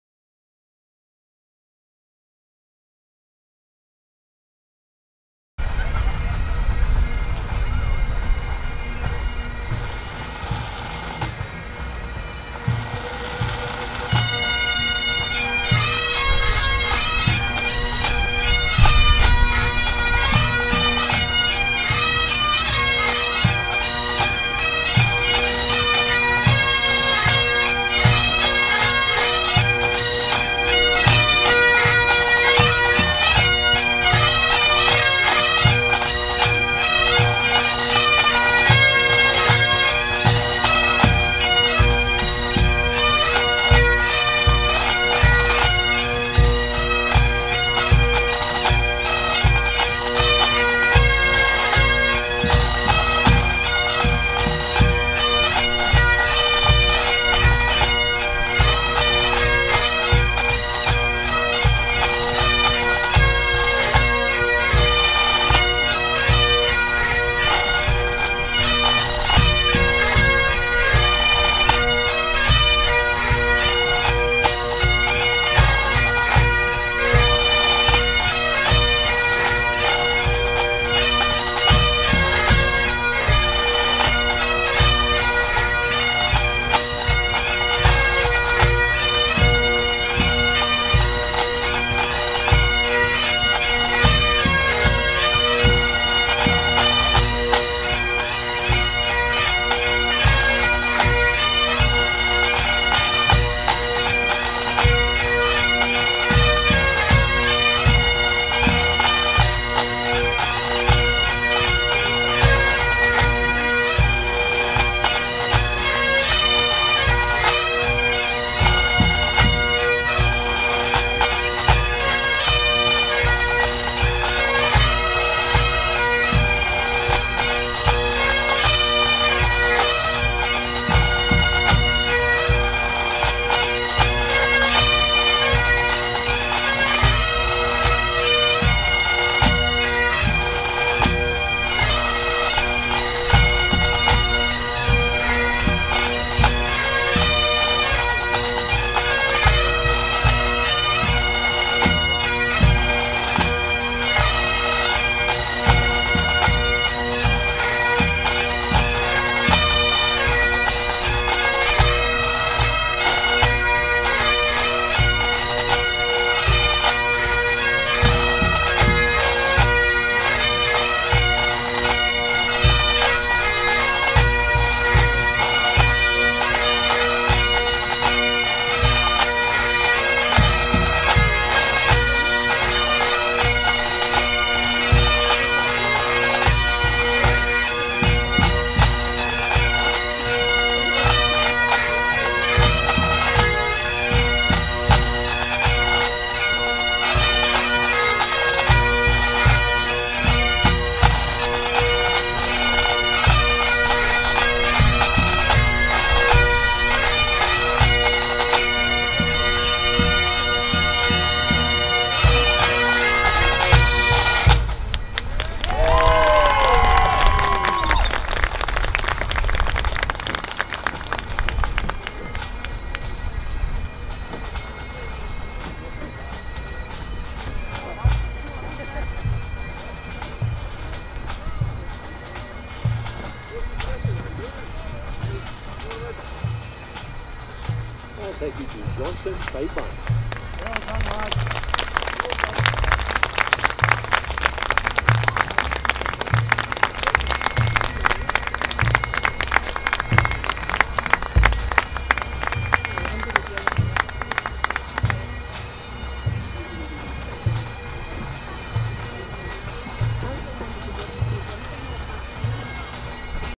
Johnstone Pipe Band, Grade 4B Worlds Qualifying 2015
4th place in qualifying ensure a plave in the World Championship Final for Johnstone.